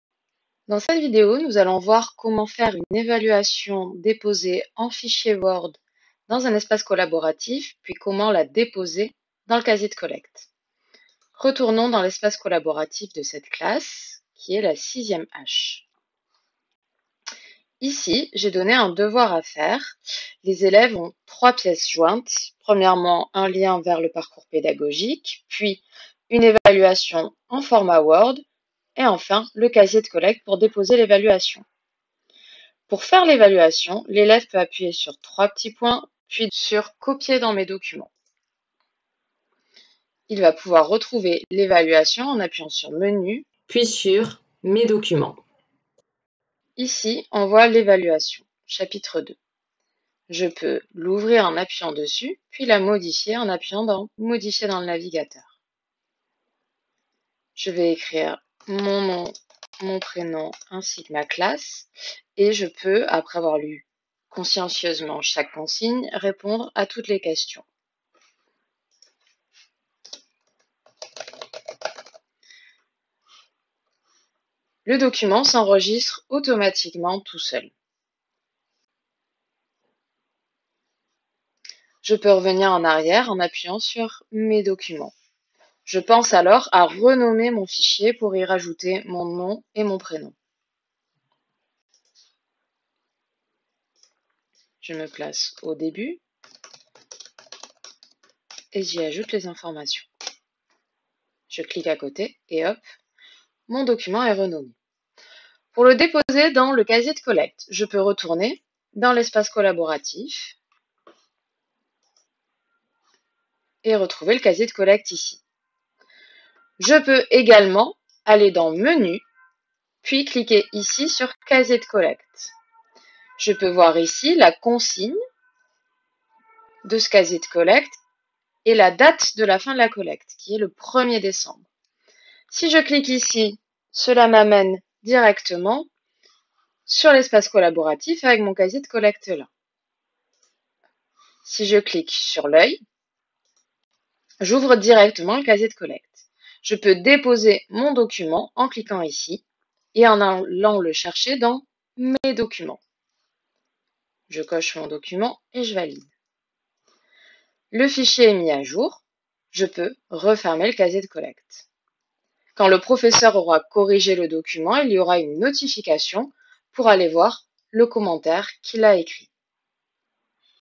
Tuto oZe : Faire une évaluation sur Word et la déposer dans un casier de collecte
Vidéo réalisée par Fanny Gaujacq, professeure-documentaliste